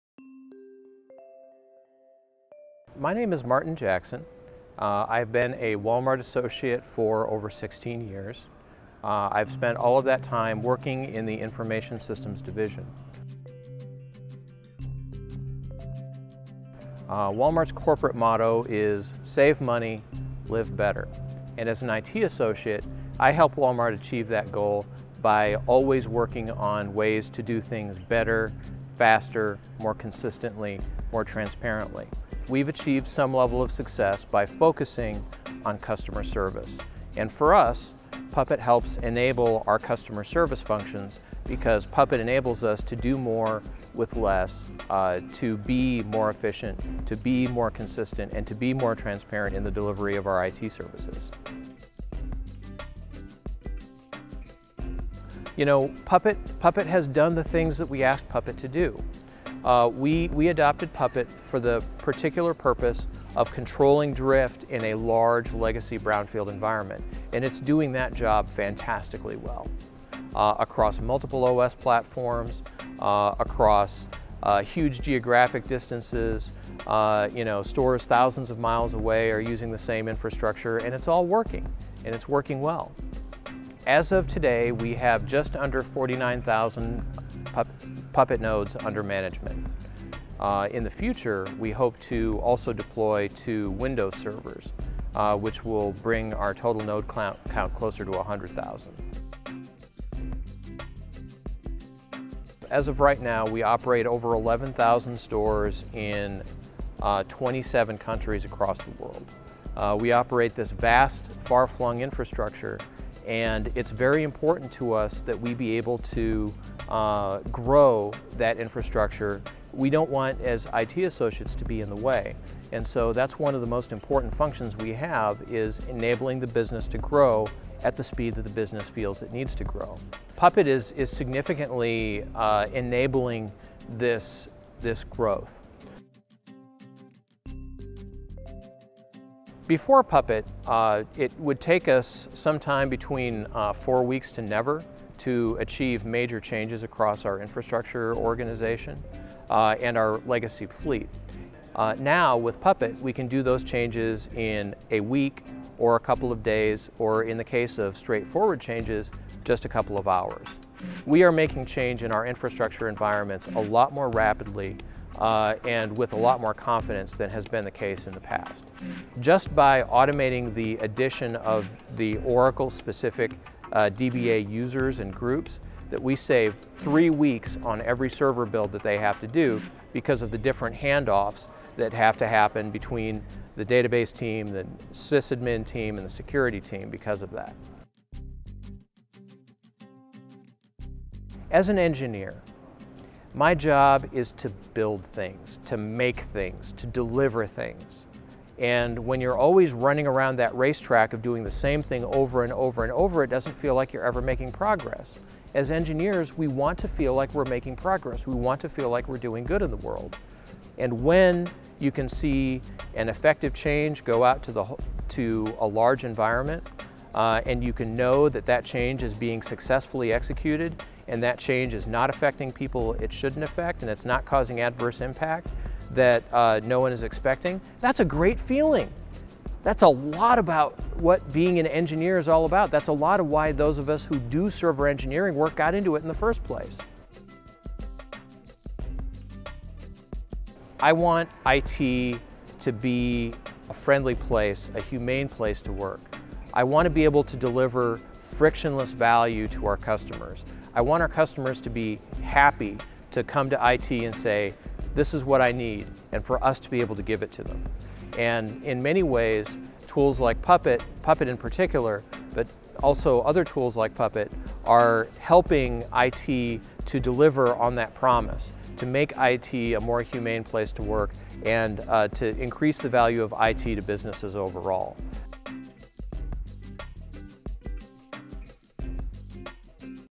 Practicing World-Class IT at Walmart with Puppet
Walmart uses Puppet to help fulfill its mission of providing goods to customers at an affordable price. Walmart is currently managing more than 49,000 servers with Puppet, in 11,000 stores located in 27 countries. Before Puppet, it would take four weeks to never to implement changes across its vast infrastructure; now it can take just a couple of days, or in some cases, a couple of hours.